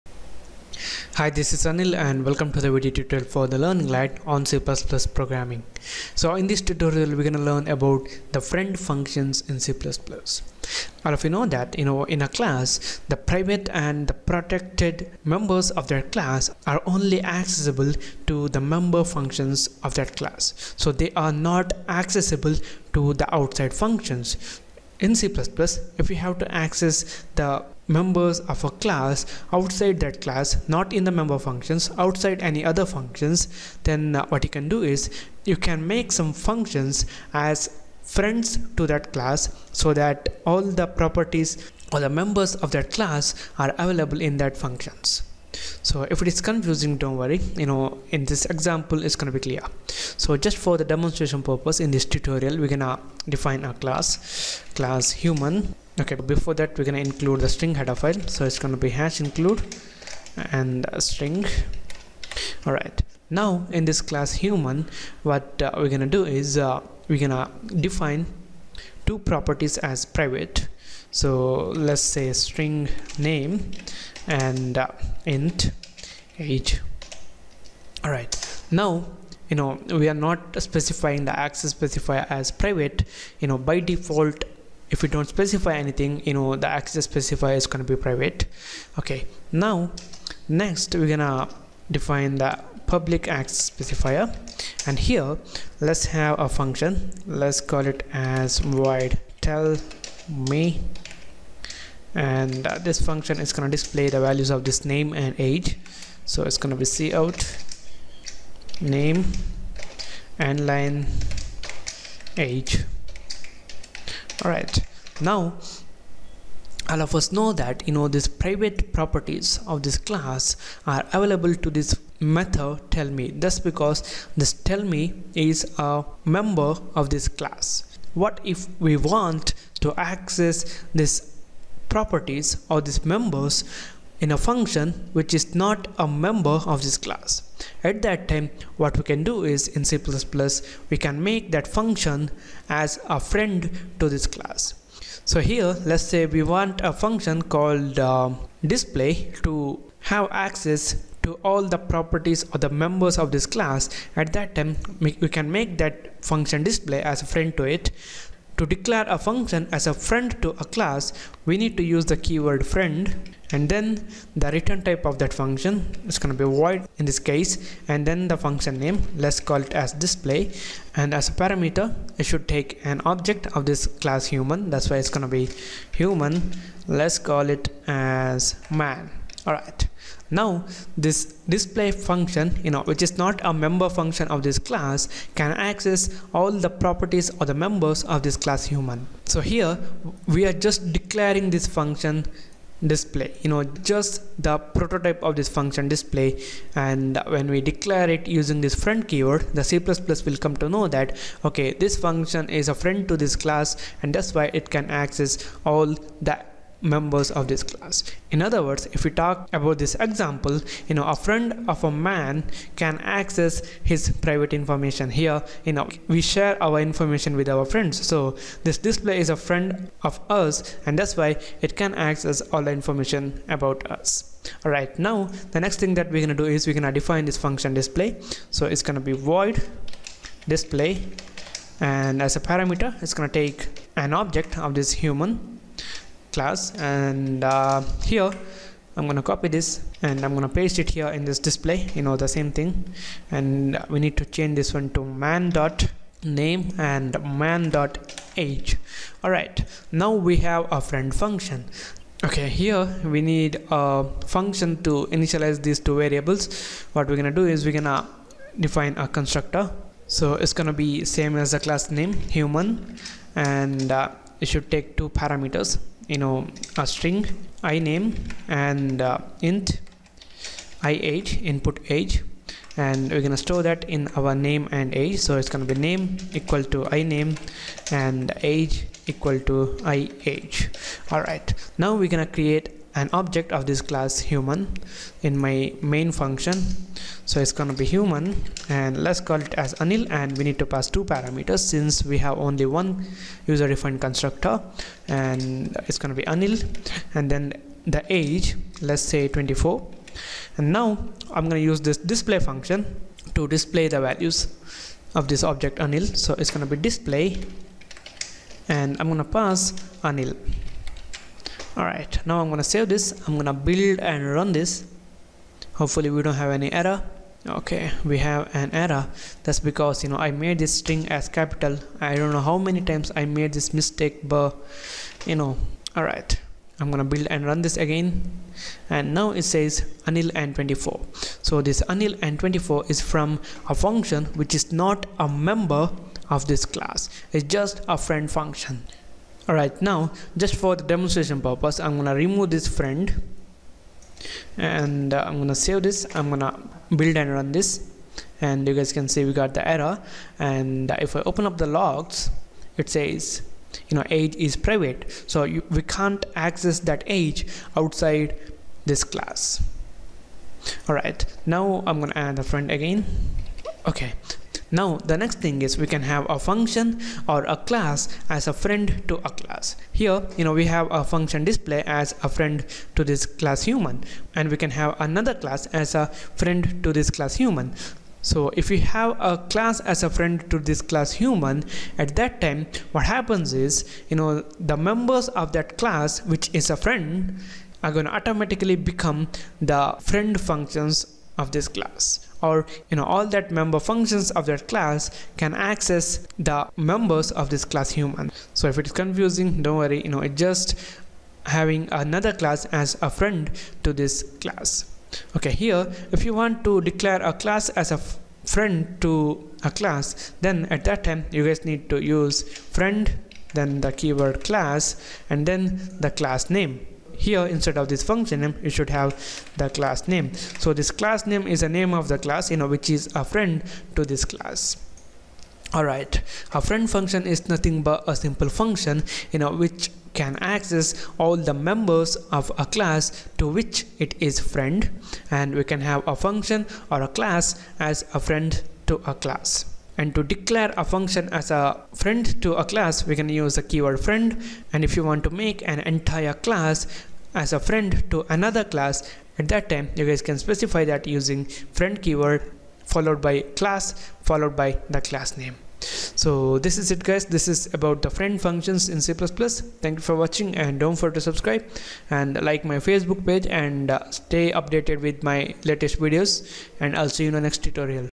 Friend Function | CPP Object Oriented Programming Video Tutorial
In this cpp Object Oriented programming video tutorial, you will learn about the friend function with example.

You are gonna learn what is a friend function, how to declare a it, how to define it, how to access the properties or members of a class in detail with example.